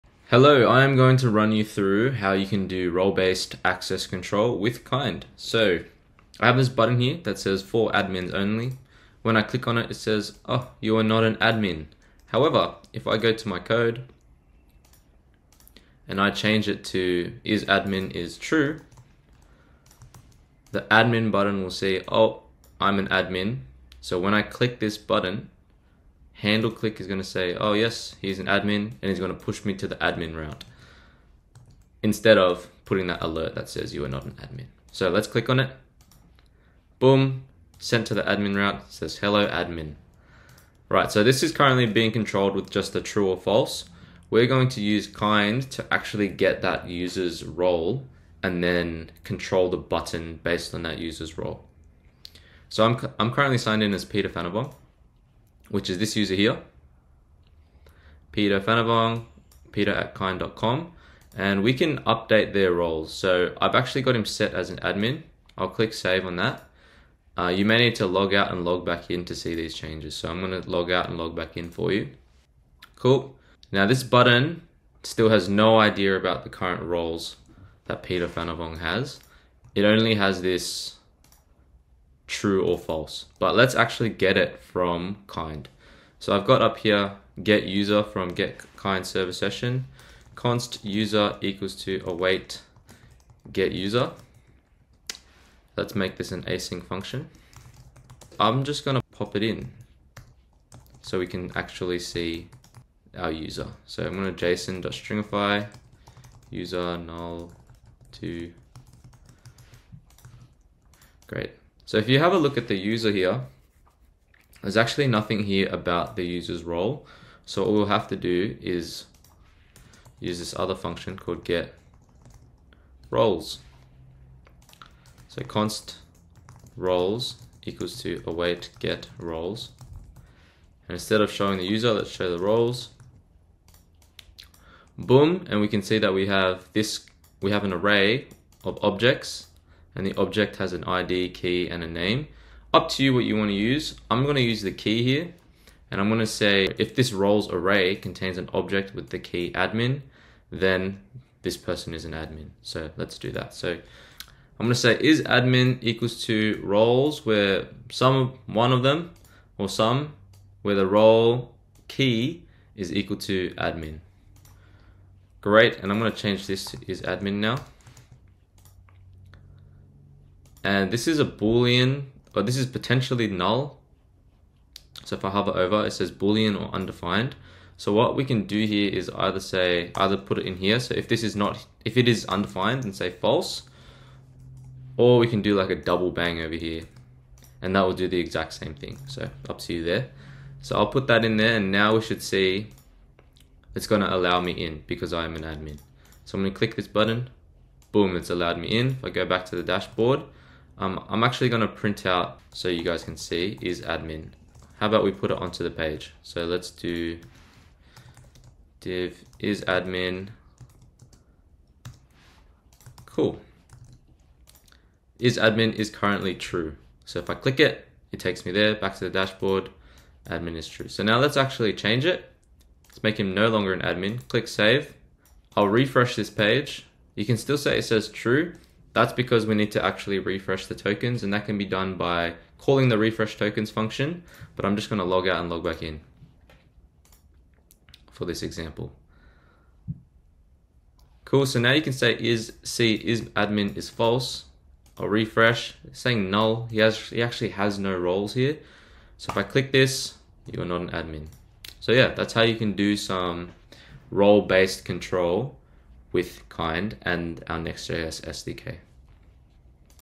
Intro to role-based access control (RBAC) with Kinde
Peter (Software Engineer @ Kinde) showcases how to use role-based access control within your Kinde application.